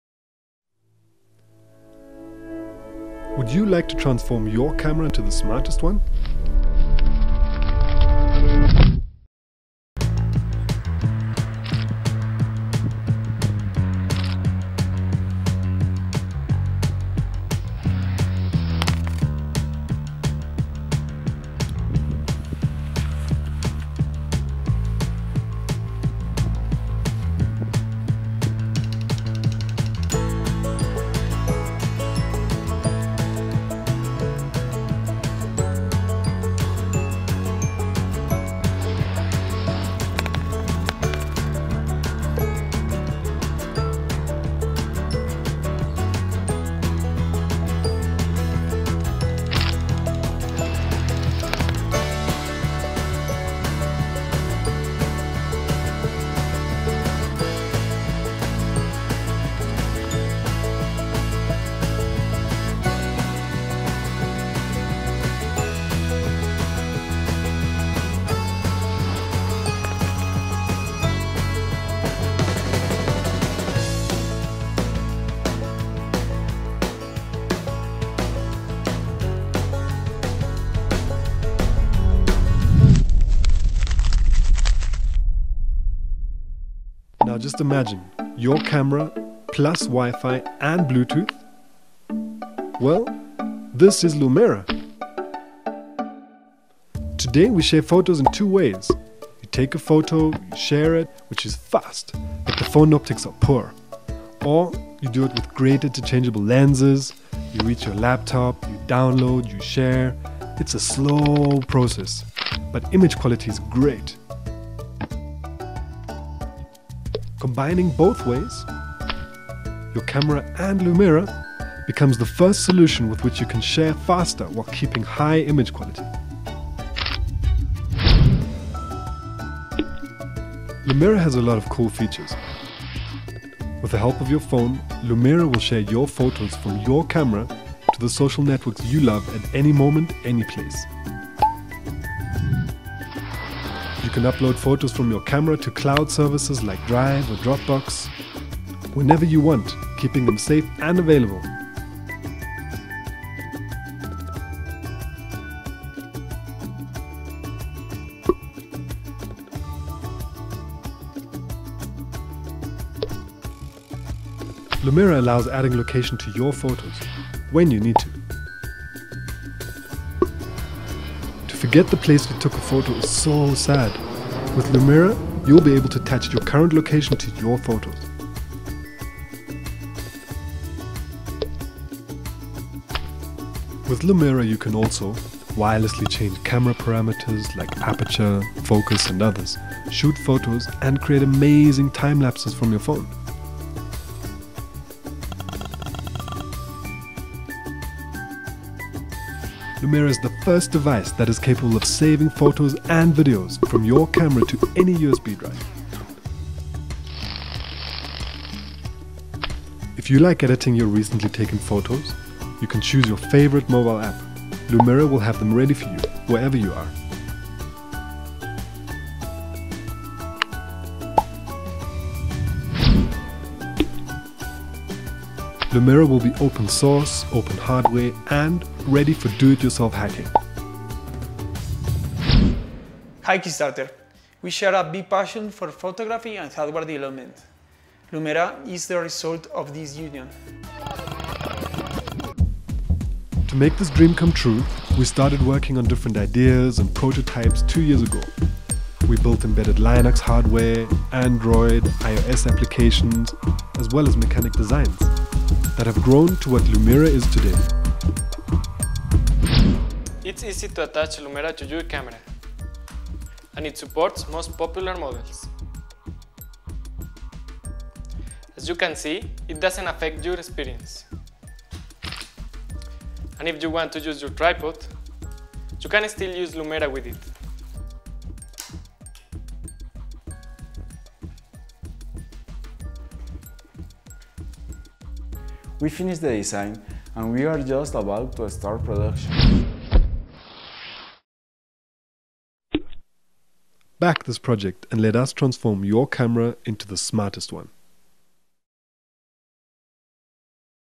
Lumera: Transform your camera into the smartest one
Lumera is the first device that allows users to one-click share photos from camera and make backups on the go without taking phone out of the pocket. This is accomplished by pre-configuring preferred social networks (like Instagram, Facebook and Twitter) with phone and with the help of its friendly OLED display and contextual buttons. This also allows users to control sharing process and visualize status.
Lumera adapts to most popular cameras like Canon and Nikon. It communicates with cameras through USB connection and will not affect users experience because it'll be attached to the camera body with a tripod thumb screw.It also allows easy connection to cloud storage services like Dropbox or Google Drive and physical storage devices like flash drives or external hard disks.